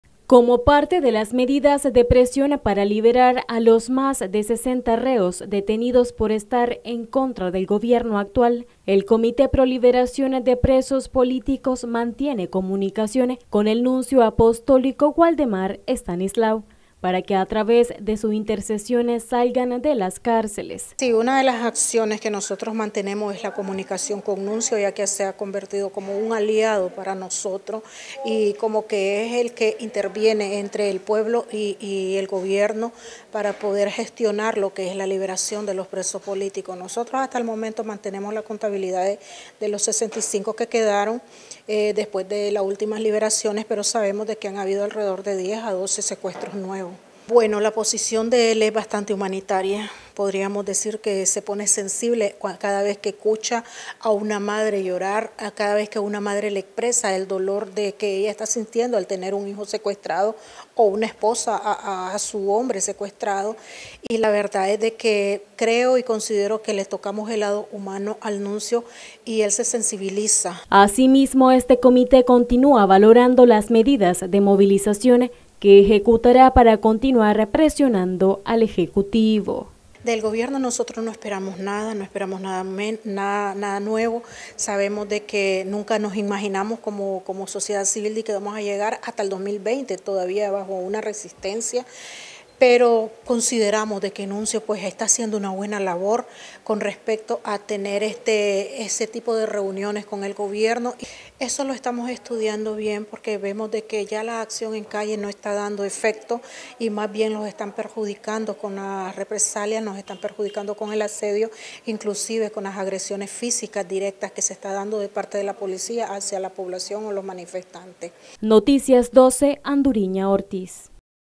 CPLPP MANTIENE COMUNICACIÓN CON EL NUNCIO
El Comité pro Liberación de Presos Políticos mantiene comunicación con él nuncio apostólico Waldemar Stanislaw, para que a través de su intercesión el gobierno libere a los más de 60 reos que se encuentran en cárceles del país.